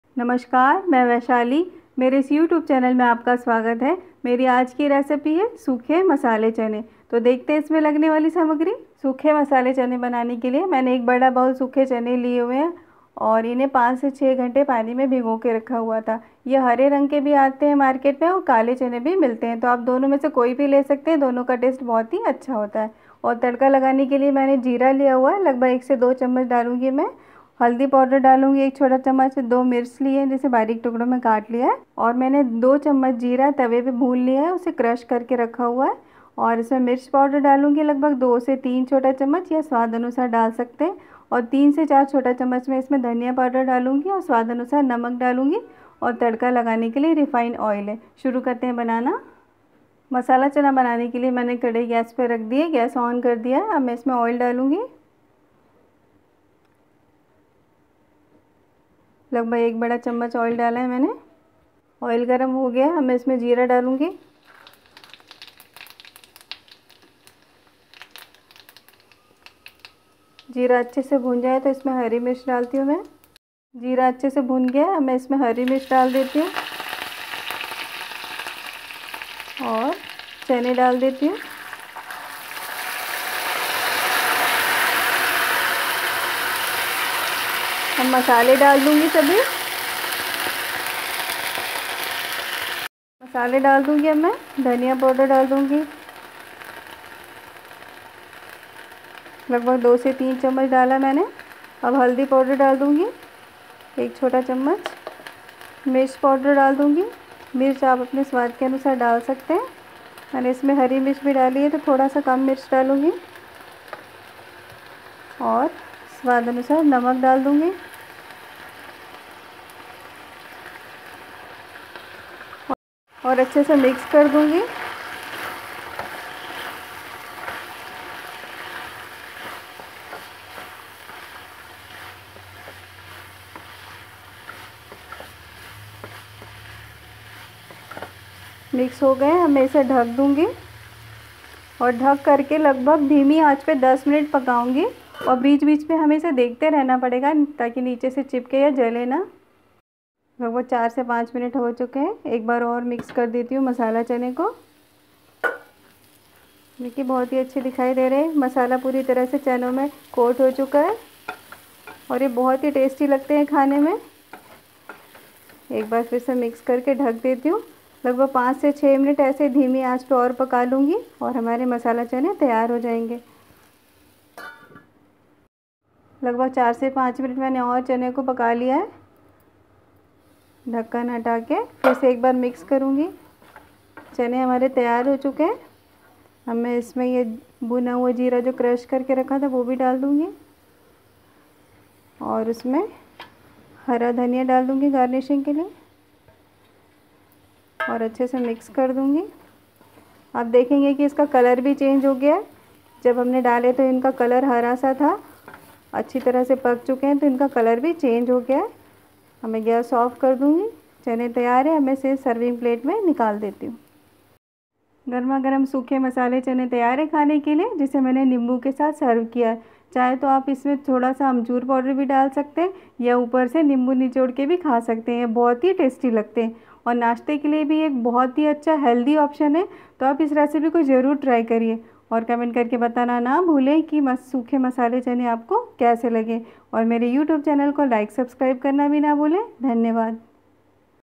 प्रसाद वाले सूखे चने बनाने की रेसिपी | Hara Chana Masala Recipe | Dry Black Chickpeas Recipe
काले/हरे चने की सब्जी बनाने की विधि, चने की सब्जी कैसे बनायें,चने की मसालेदार सब्‍जी रेसिपी बनाना देखें इस वीडियो मे |